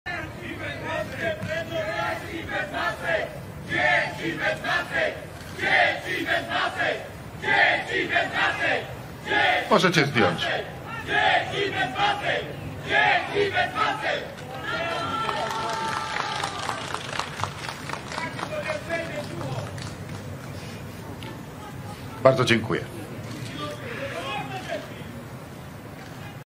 Andrzej Duda pozwala zdjąć maski dzieciom na świeżym powietrzu XD KATOWICE
Wielki Człowiek, filantrop Andrzej Duda pozwala zdjąć maski swoim podwładnym podczas swojego przemówienia.
2021r. Dudoryzowane
źródło: FB Andrzeja DUDY